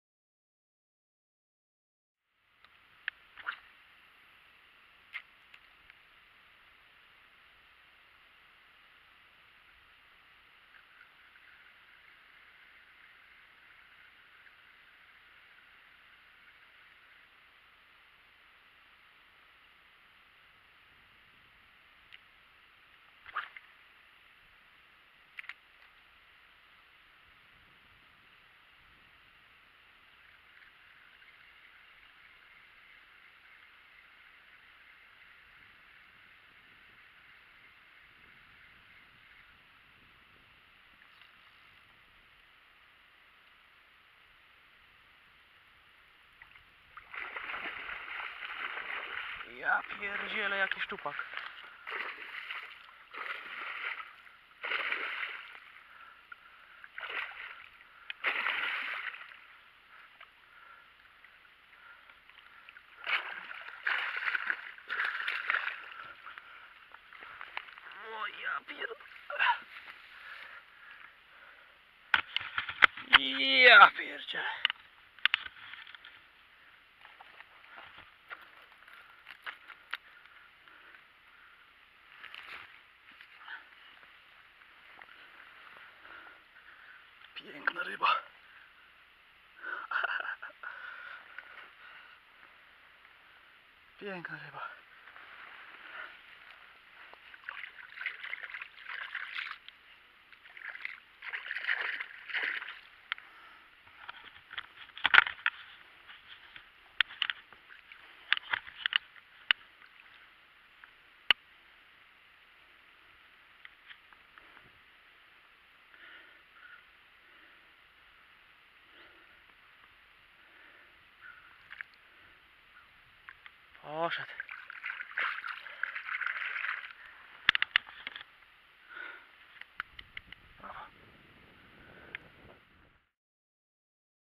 Szczupak zamiast bolenia Asp Pike Fishing
Niespodziewany przyłów w postaci szczupaka sensownych rozmiarów dziwi, zwłaszcza w związku z zauważalnym brakiem (spadkiem liczebności) tej ryby w polskich wodach płynących.